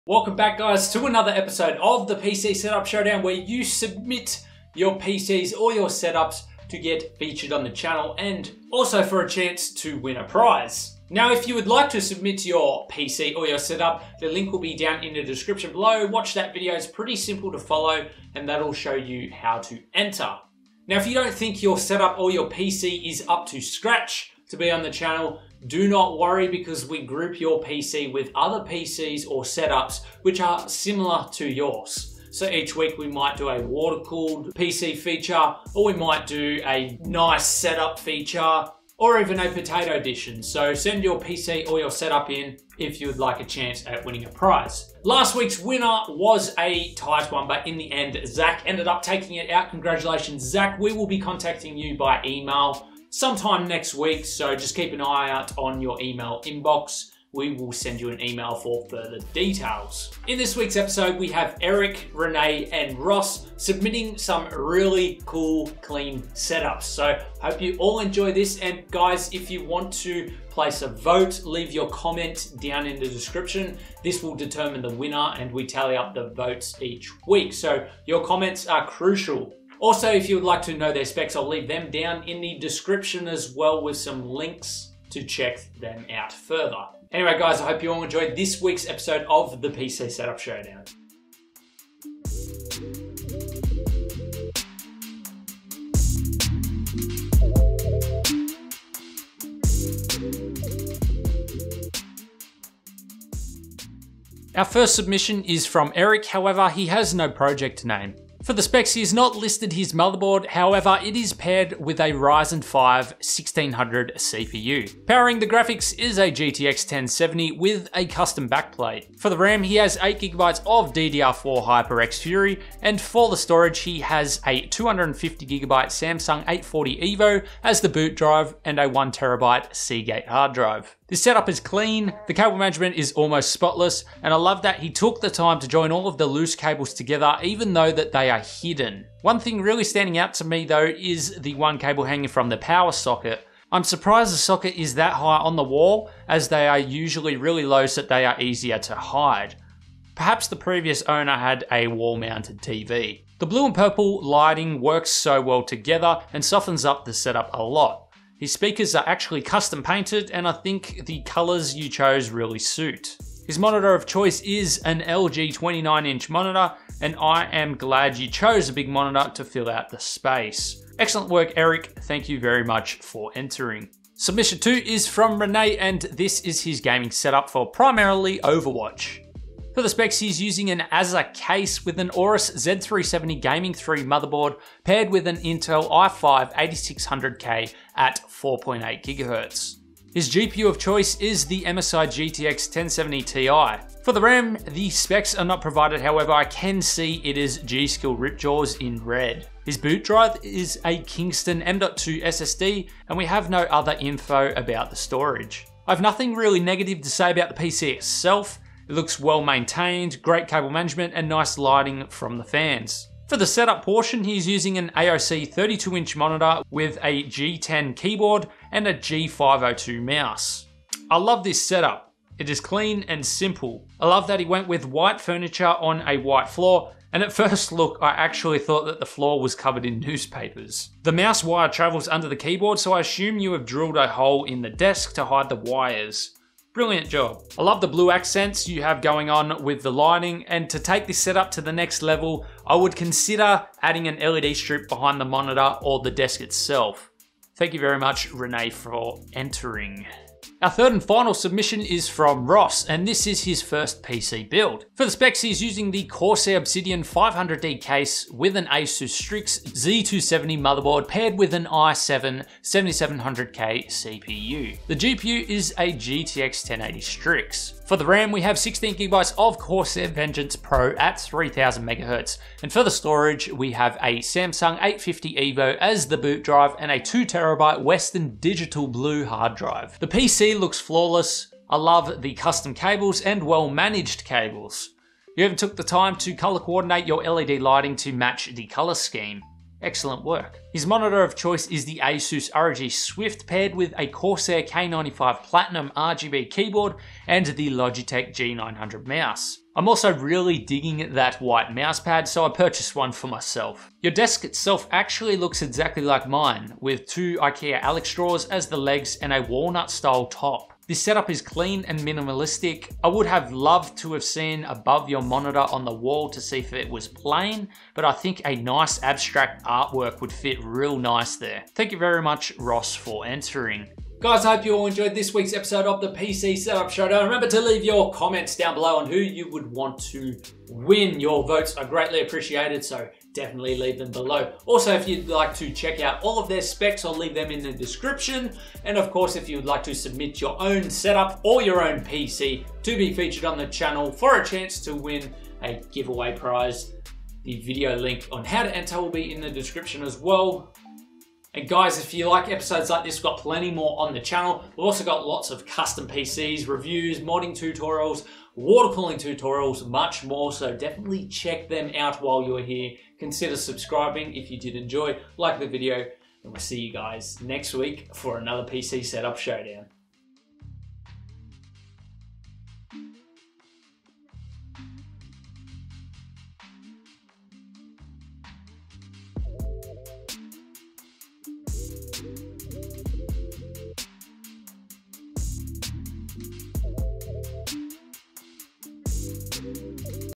PC Setup Showdown Episode 9 - ULTIMATE Gaming Setup Build Edition
PC Setup Showdown Episode 9 - ULTIMATE Gaming Setup Build Edition. In todays episode of the pc setup showdown we take a look at 3 ULTIMATE gaming setups and how clean you can make your gaming space. Each setup has its own unique style whether it be led lighting or fancy wall paper.

SPECS: ERIK
Ryzen 5 1600
GTX 1070 Armor OC
8gb of ddr4 hyperx fury ram
250gb Samsung 840 evo ssd
1tb seagate hdd
Razer blackwidow v2
LG 29wk500

SPECS: RENE
I5 8600K 4,8GHz
Aorus Intel Z370 Gaming 3 Motherboard
Arctic Freezer 33 e sports edition cpu cooler
Seasonic Focus Plus Gold 650 Watt PSU
MSI GTX 1070ti GPU
6x Enermax T.B RGB Case Fans
Azza Thor Inferno Case
Logitec G910 Orion Spectrum Keyboard
G502 Gaming Mouse
G933 Arthehmhis Spectrum Wirless headset
Z-623 Logitech THX Soundsystem
AOC Agon AG322QCX Monitor

SPECS: ROSS
i7 7700k
Asus Strix z270e
16gb Corsair Vengeance Pro 3000hz
GTX 1080 Strix
Corsair H150i AIO
Samsung 850 evo 500gb
WD Blue 2TB
EVGA 750w G3 PSU
Corsair Obsidian 500D SE
6x Corsair LL120 Fans
Commander Pro + 3 LED Strips
Asus ROG Swift PG278QR Monitor
Corsair ST100 Headphone Stand
Corsair HS70 Wireless Headset
Corsair K95 Platinum RGB Keyboard
Logitech G900 Mouse
Glorious PC Extended Mouse Mat

SUPPORT US:
AU Affilate Amazon